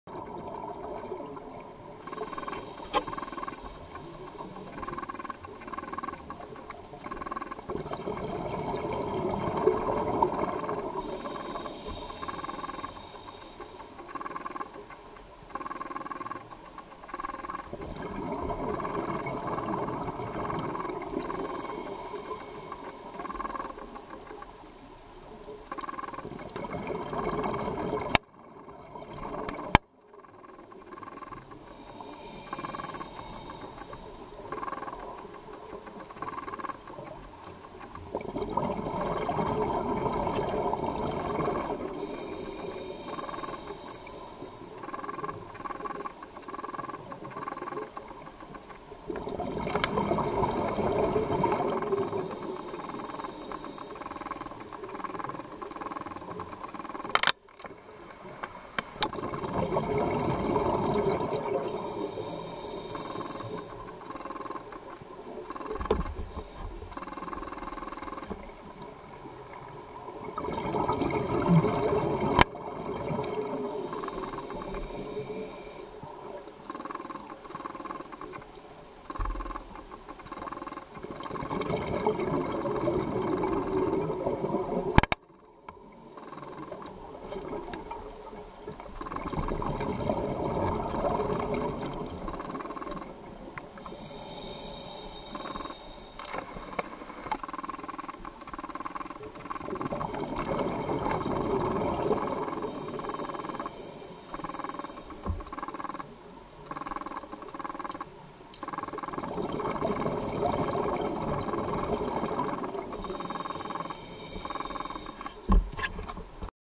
Courting Ocean triggerfish
We saw 20-25 Ocean Triggerfish dancing off Jack McKinny's canyon in Grand Cayman. Interestingly you can hear their clicks on the video. My guess is that we were pretty lucky to see this (even though we missed the reef sharks).